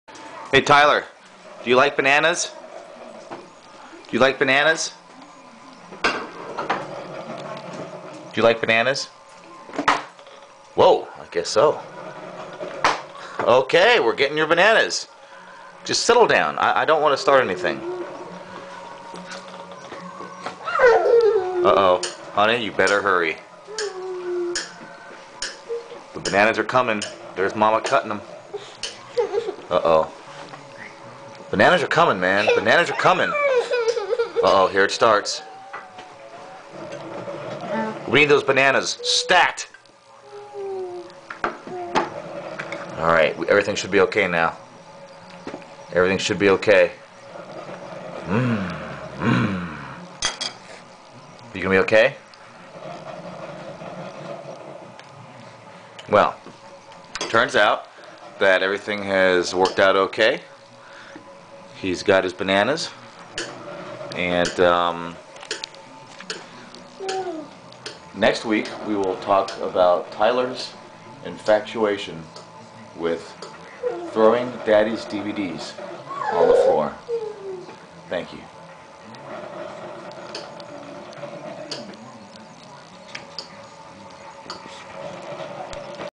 Tyler loves bananas!
My little boy loves bananas! This short video I made shows this.